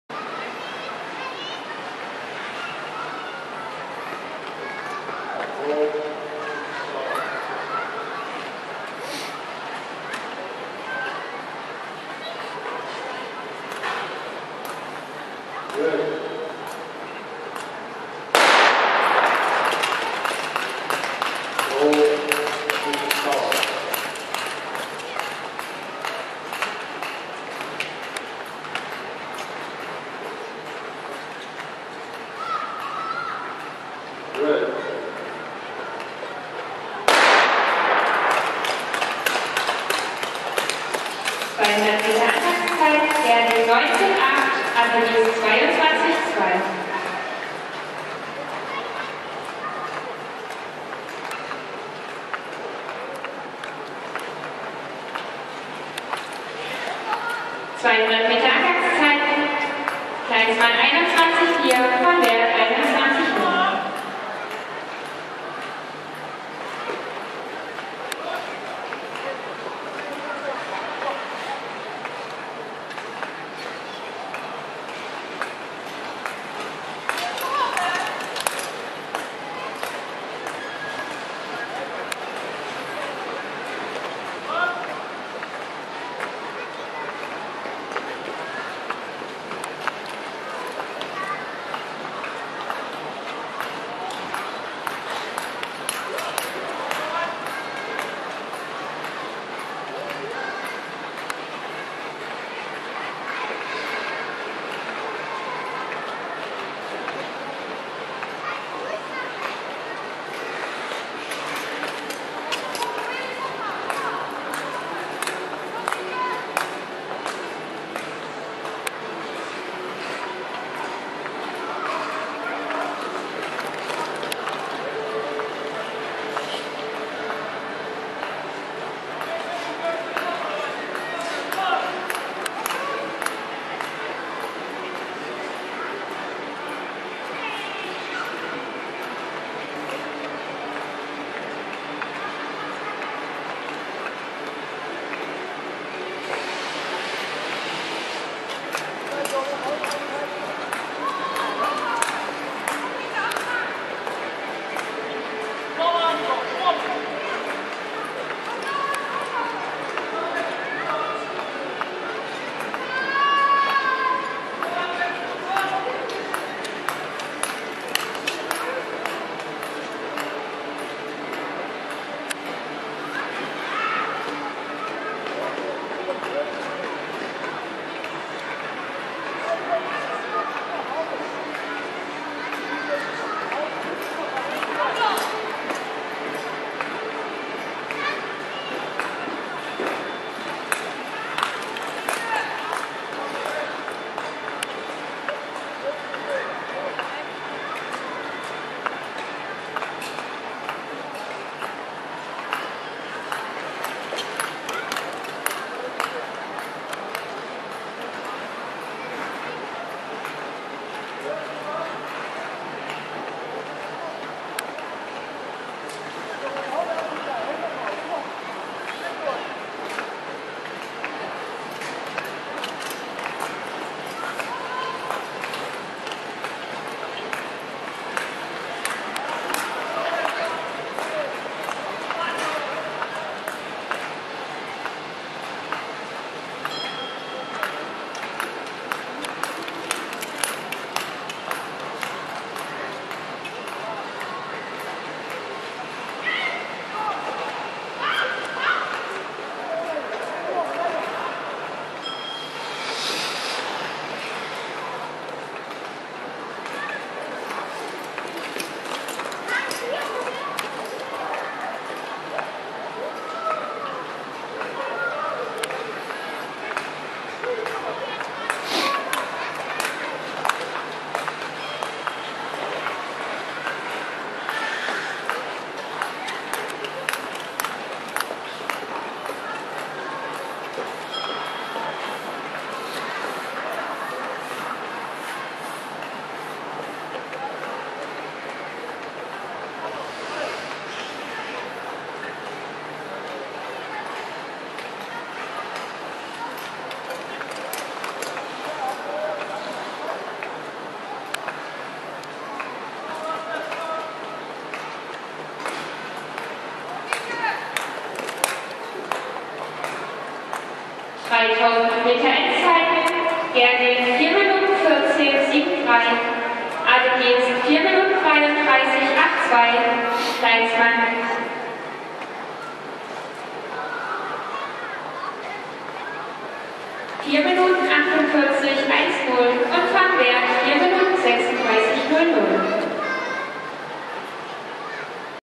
3000 m Erfurt
3000 m in Erfurt
Thomas Geerdinck - Loes Adegeest
Lotte van Weert - Nienke Kleinsman